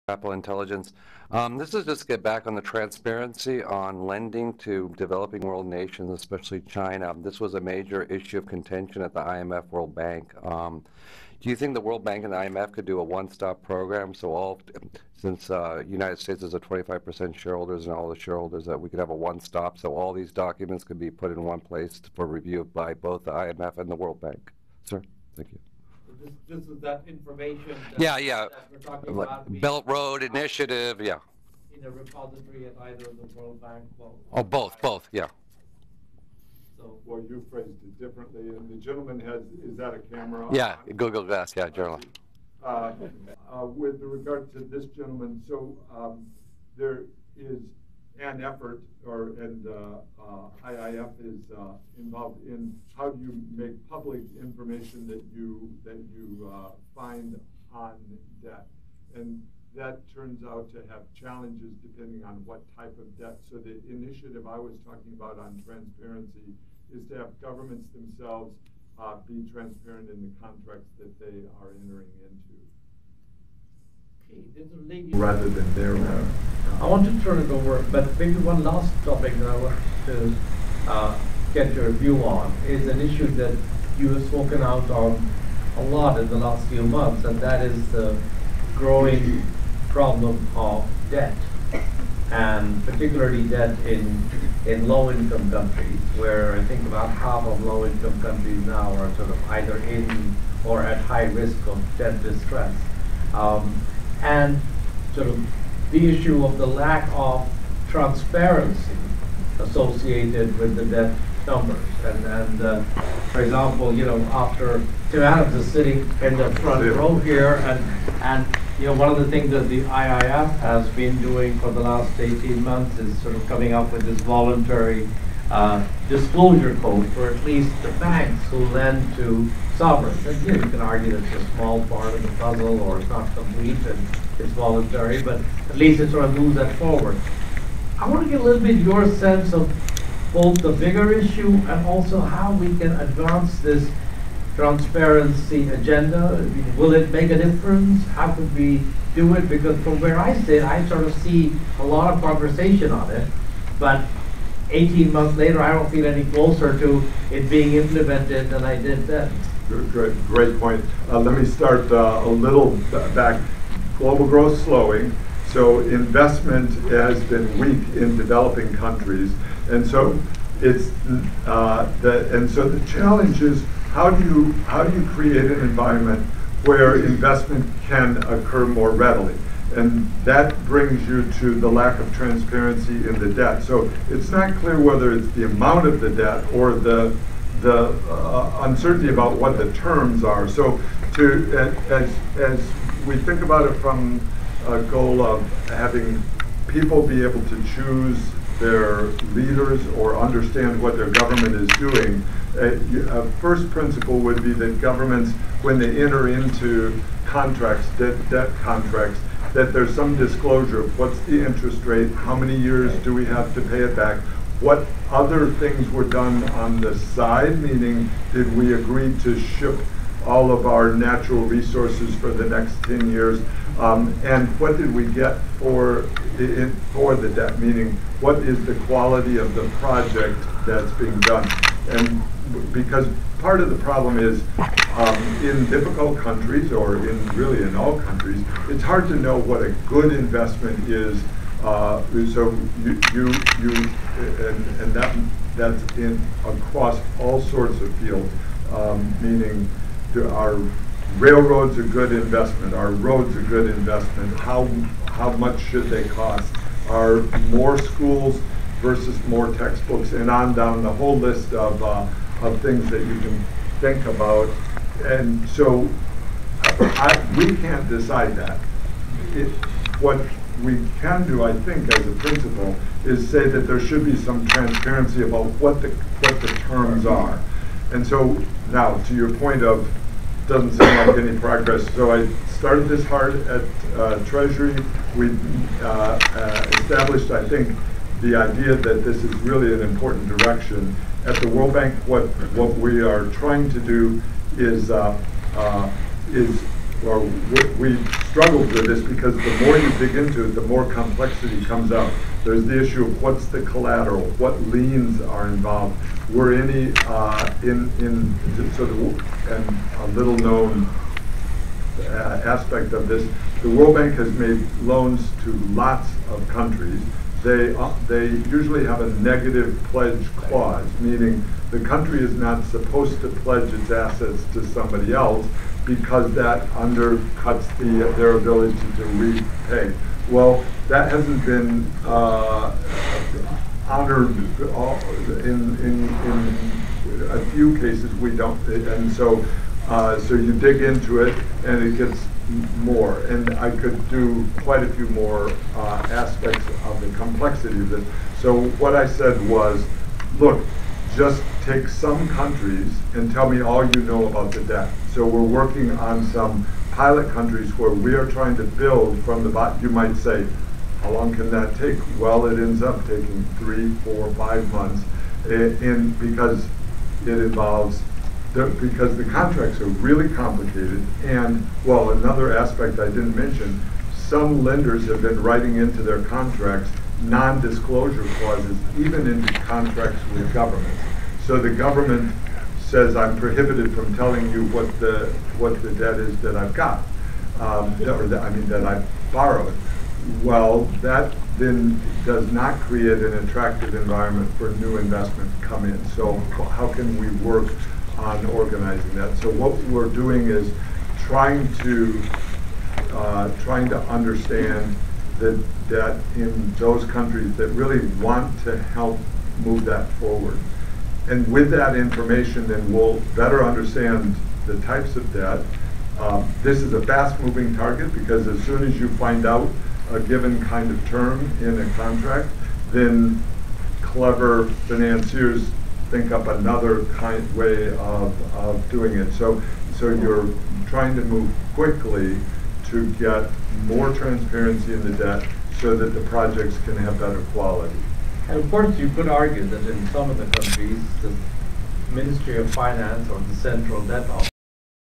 World Bank President David Malpass pushes for Chinese loan transparency
World Bank President David Malpass filmed by  Capitol Intelligence/CI GCA using CI Glass on the need for transparency in Chinese loans to developing countries at Center for Global Development panel "A Conversation with David Malpass"  moderated by CGD President Masood Ahmed in Washington, DC.  November 5,  2019.